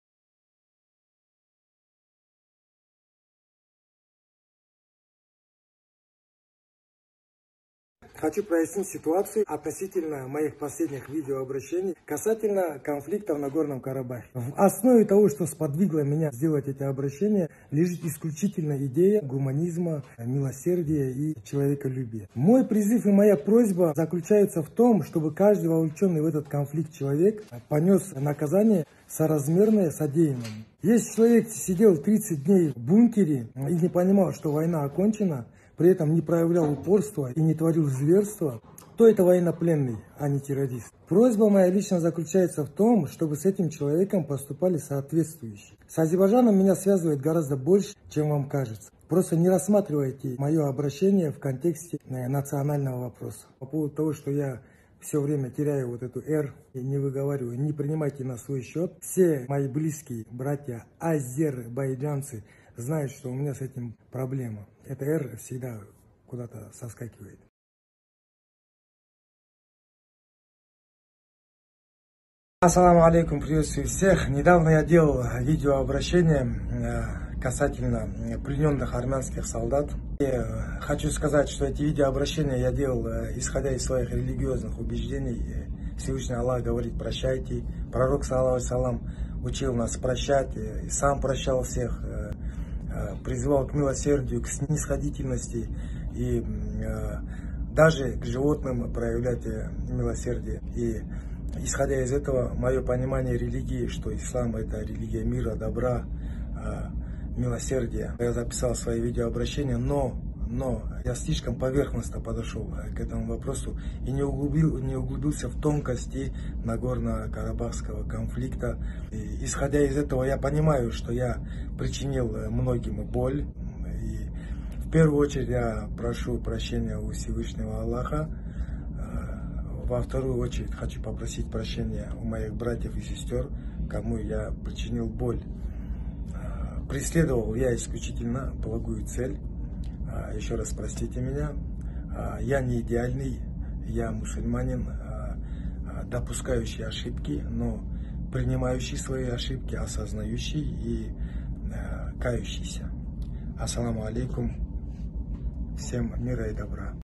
Магомед Исмаилов извинился перед азербайджанцами.
Магомед Исмаилов извинился перед Аллахом в первую очередь и азербайджанцами. За то что не углубился в тонкости нагорно Карабахского конфликта.